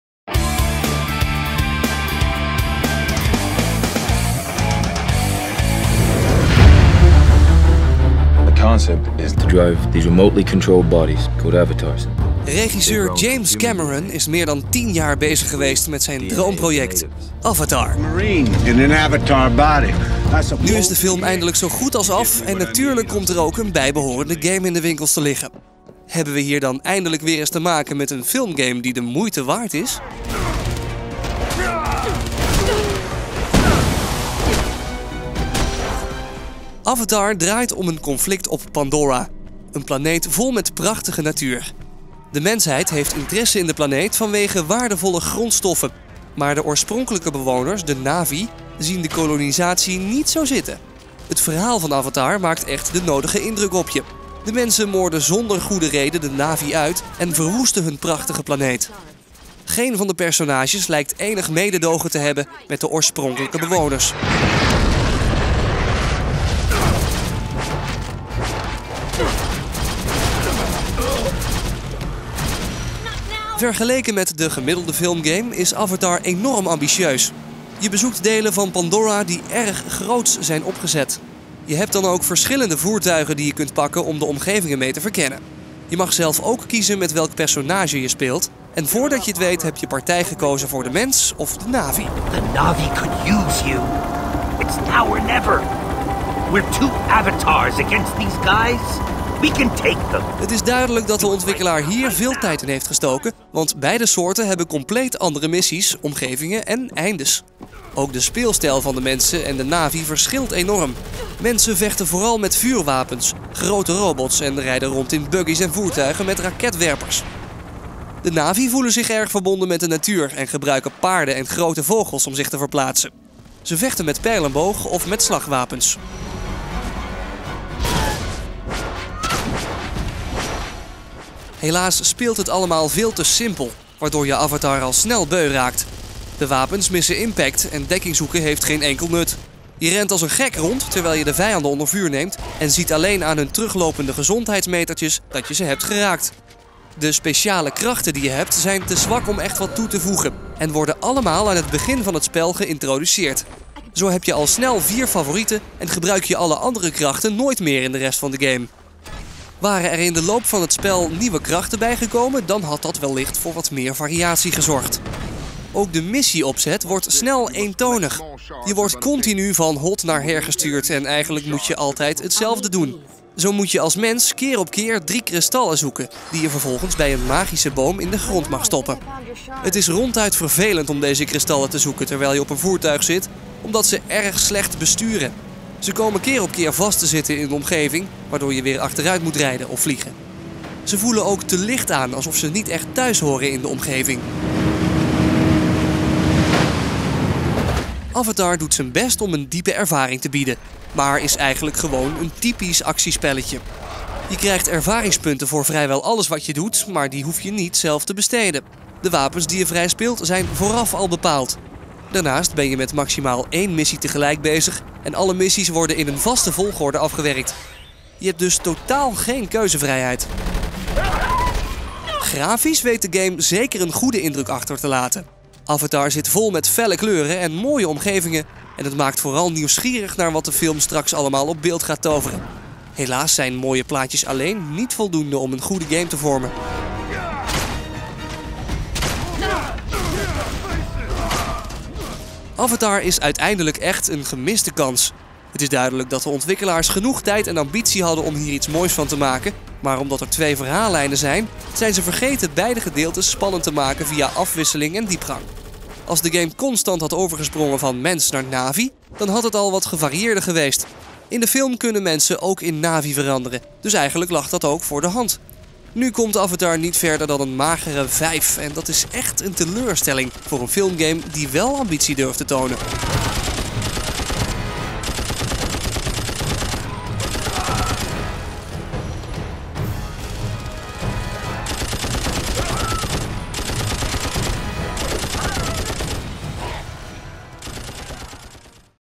James Cameron's Avatar: The Game - Video Review
Gamer.nl bespreekt James Cameron's Avatar: The Game